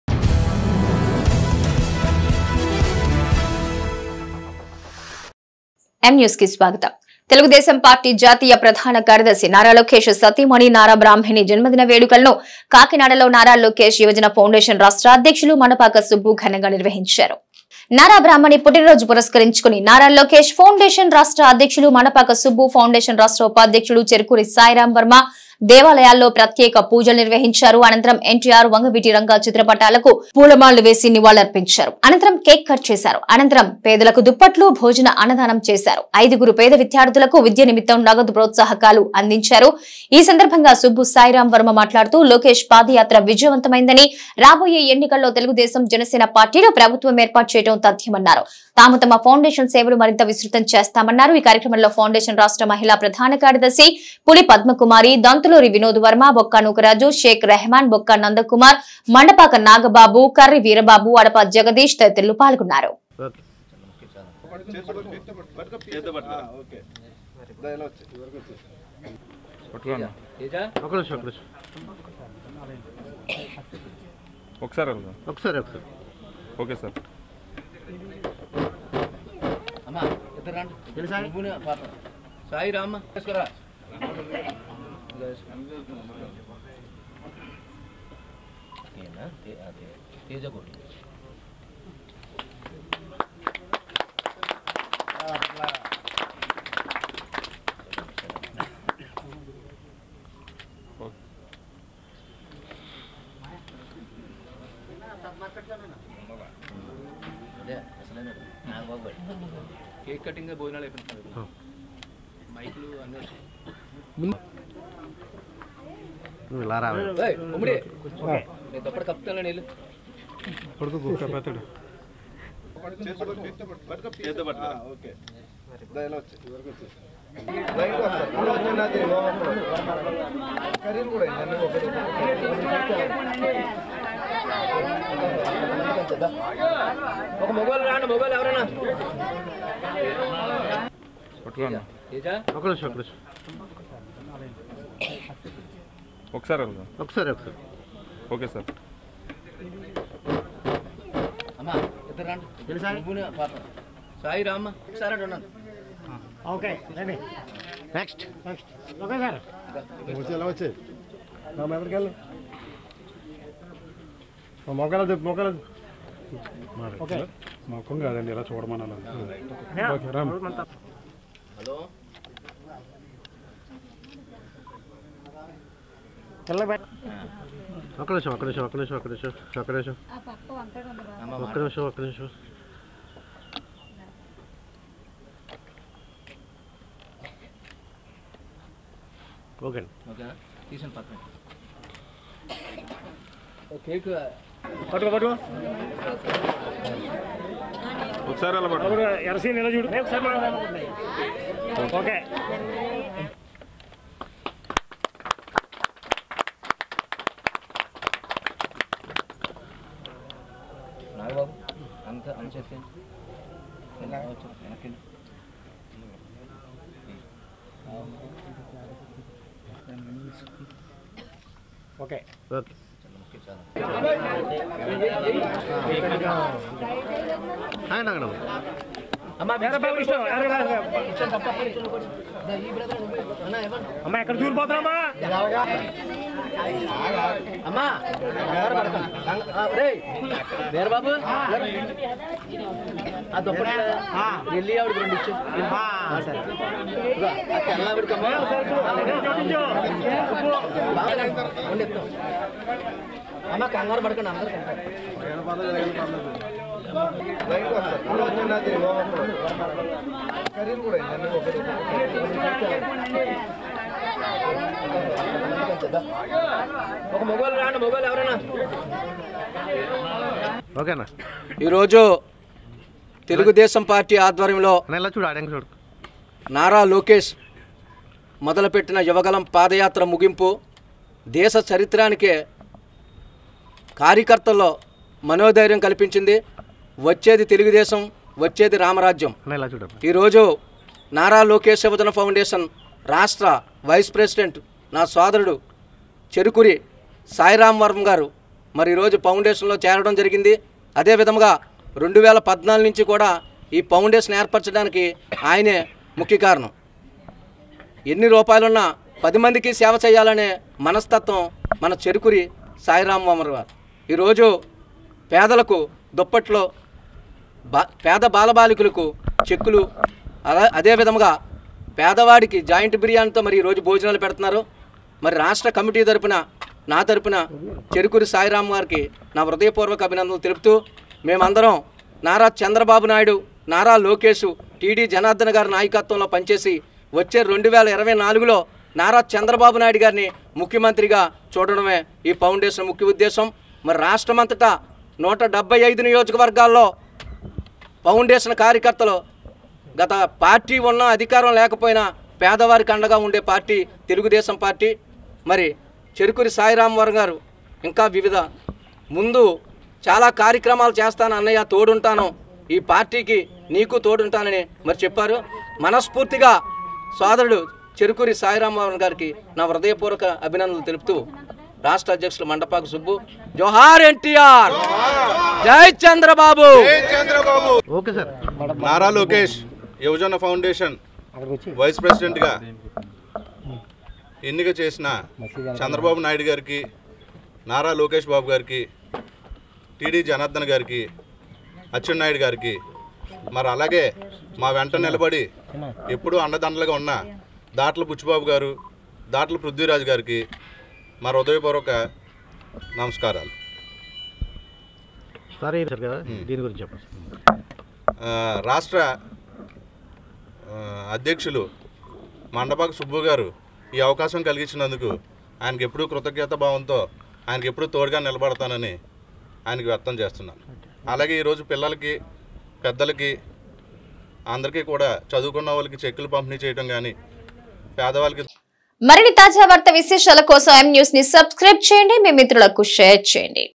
నారా లోకేష్ సతీమణి బ్రహ్మిణి జన్మదిన వేడుకలను ఘనంగా|మండపాక సుబ్బు, సాయిరామ్ వర్మఆధ్వర్యంలో ||M NEWS
తెలుగుదేశం పార్టీ జాతీయ ప్రధాన కార్యదర్శి నారా లోకేష్ సతీమణి నారా బ్రహ్మిణి జన్మదిన వేడుకలను కాకినాడలో నారా లోకేష్ యువజన ఫౌండేషన్ రాష్ట్ర అధ్యక్షులు మండపాక సుబ్బు  ఘనంగా నిర్వహించారు. వాయిస్: నారా బ్రాహ్మణి పుట్టినరోజు పురస్కరించుకుని నారా లోకేష్ ఫౌండేషన్ రాష్ట్ర అధ్యక్షులు మండపాక సుబ్బు, ఫౌండేషన్ రాష్ట్ర ఉపాధ్యక్షుడు చెరుకూరి సాయిరామ్ వర్మ  దేవాలయాల్లో ప్రత్యేక పూజలు నిర్వహించారు.||M NEWS TELUGU